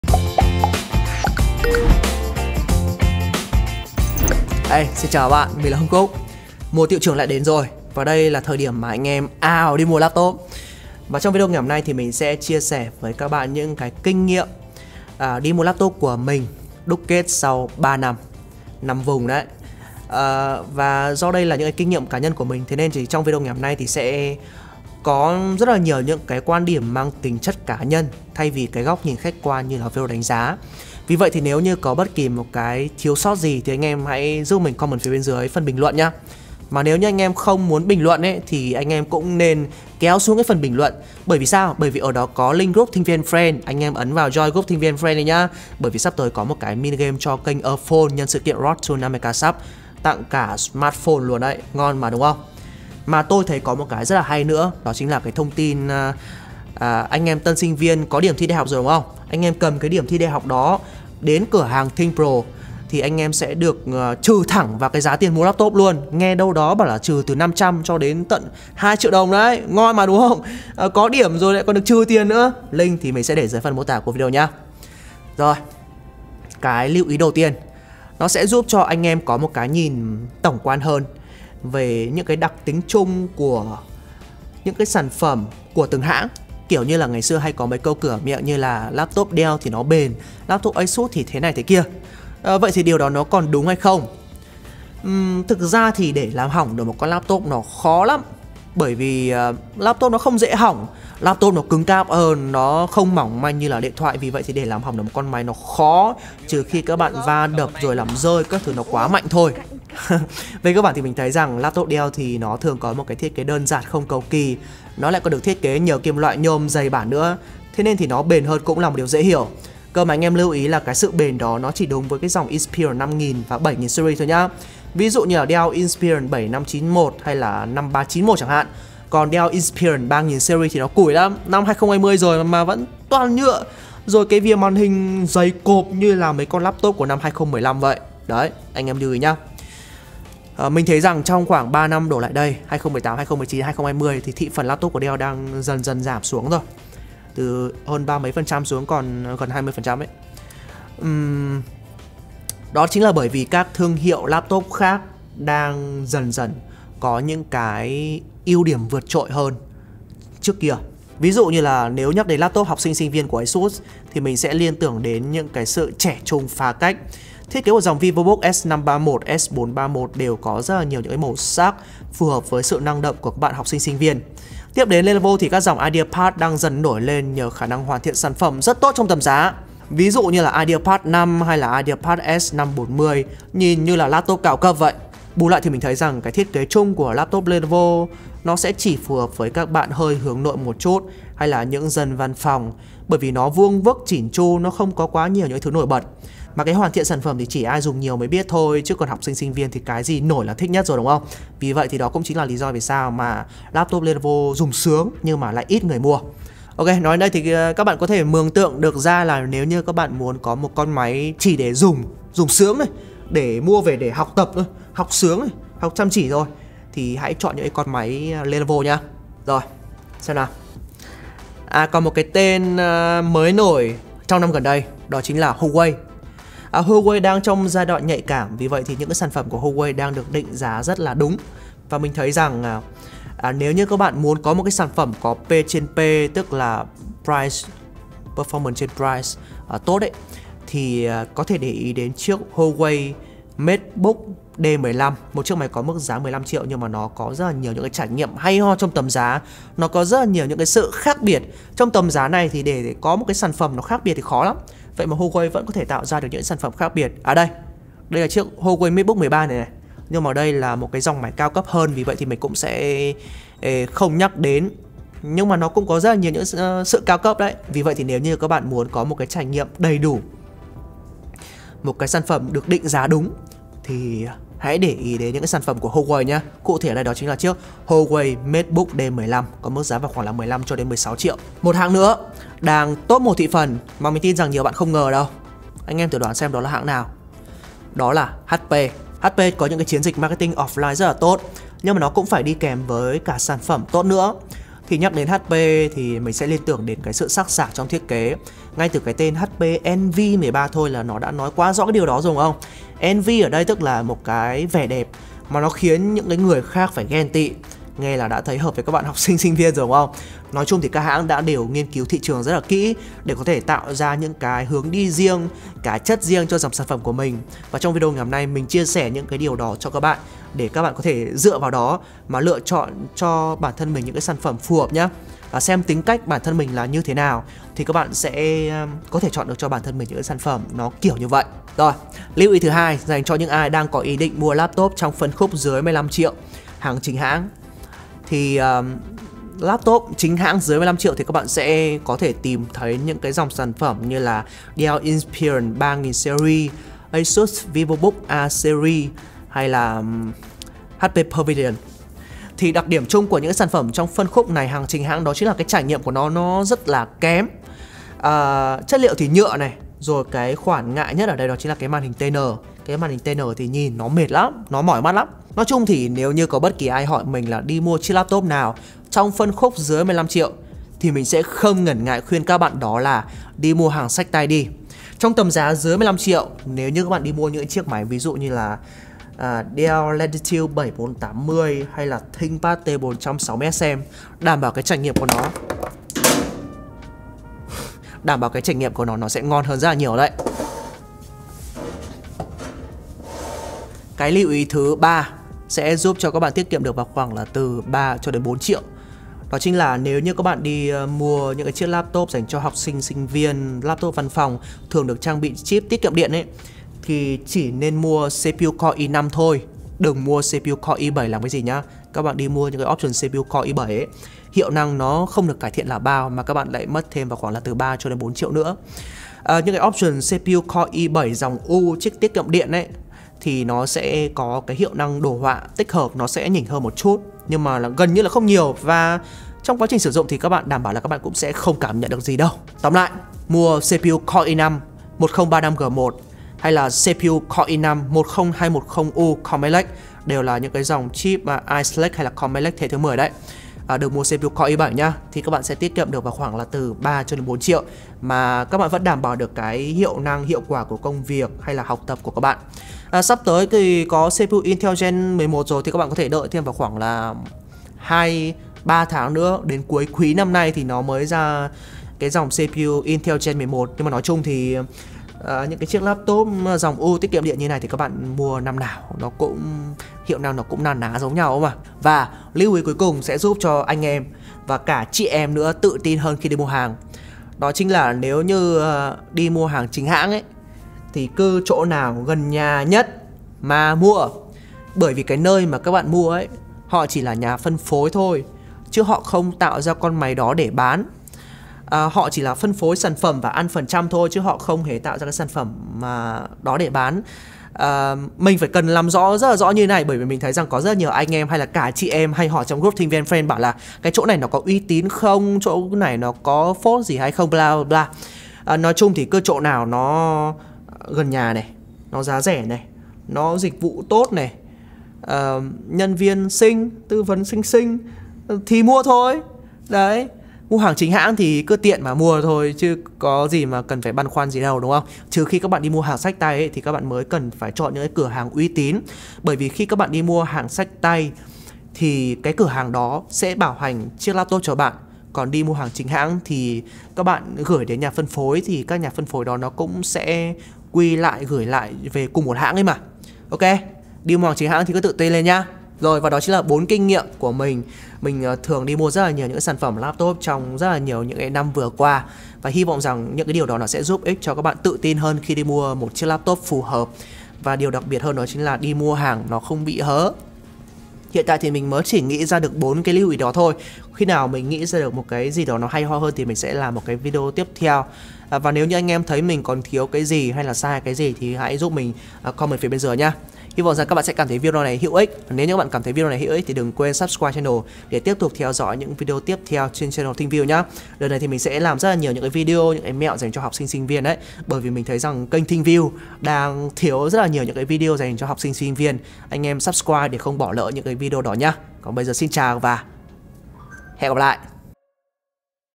Cách Chọn Laptop Cho Sinh Viên (đúc kết 3 năm)
ThinkView chia sẻ kinh nghiệm mua Laptop cho học sinh sinh viên, laptop văn phòng, laptop giá rẻ!
Follow Hưng Khúc:
ThinkView là kênh youtube về Laptop & PC thuộc hệ sinh thái truyền thông của ThinkView: